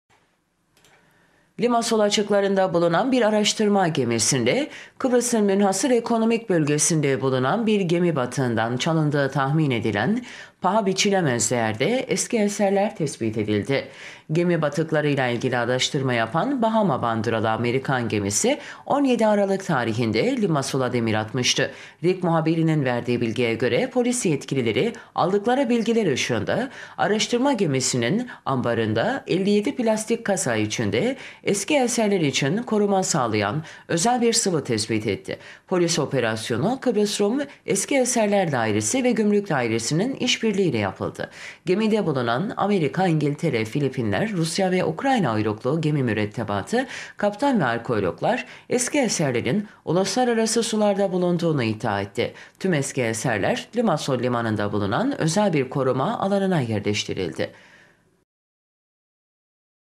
ESERLERE YAPILAN OPERASYONLA EL KONULDU
Limasol açıklarında bulunan bir araştırma gemisinde, Kıbrıs’ın Münhasır Ekonomik Bölgesi’nde bulunan bir gemi batığından çalındığı tahmin edilen, paha biçilemez değerde eski eserler tespit edildi. Gemi batıklarıyla ilgili araştırma yapan, Bahama bandıralı Amerikan gemisi, 17 Aralık tarihinde Limasol’a demir atmıştı. RİK muhabirinin verdiği  bilgiye göre, polis yetkilileri aldıkları bilgiler ışığında, araştırma gemisinin ambarında, 57 plastik kasa içinde, eski eserler için koruma sağlayan özel bir sıvı tespit etti. Gemide bulunan, Amerika, İngiltere, Filipinler, Rusya ve Ukrayna uyruklu gemi mürettebatı, kaptan ve arkeologlar, eski eserlerin uluslararası sularda bulunduğunu iddia etti. Tüm eski eserler, Limasol Limanı’nda bulunan özel bir koruma alanına yerleştirildi.